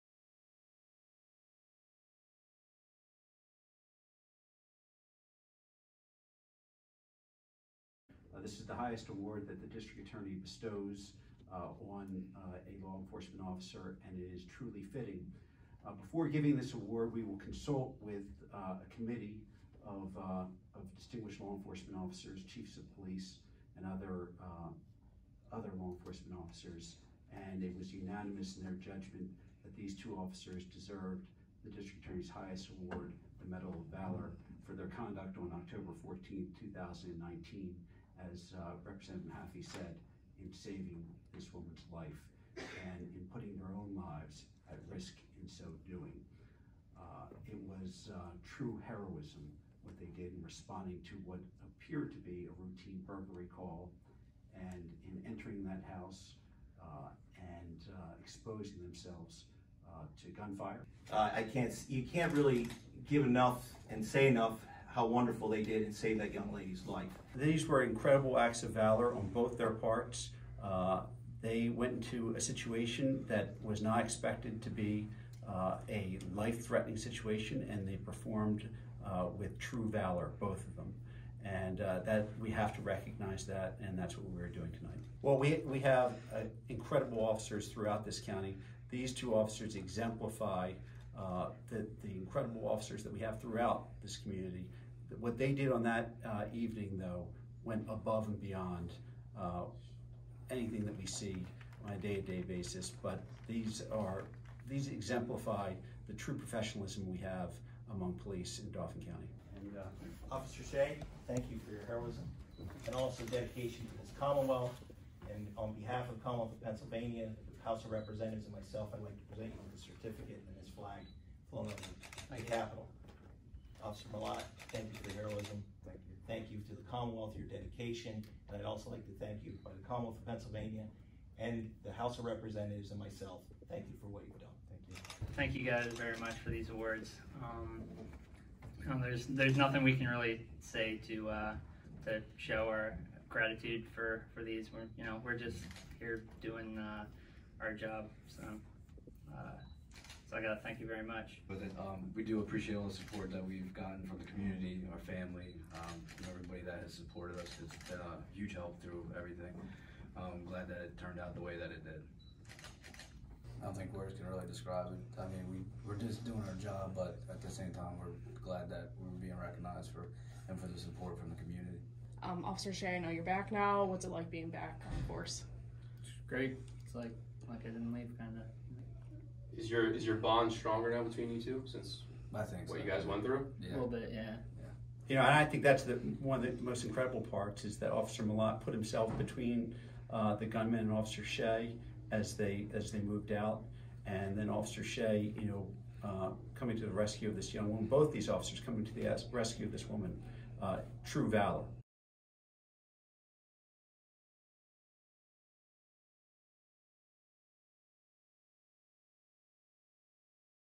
Lower Swatara Township Police Medal of Valor presentation
Lower Swatara Township officers Timothy Shea and Joshua Malott receive Dauphin County's Medal of Valor for their role in saving a woman during a standoff with a barricaded gunman on Oct. 14, 2019.

Shea suffered a gunshot wound to the leg, and officials say Malott positioned himself in front of Shea to shield the other officer during the standoff. They were able to get themselves and the woman to safety, and the shooter was unharmed.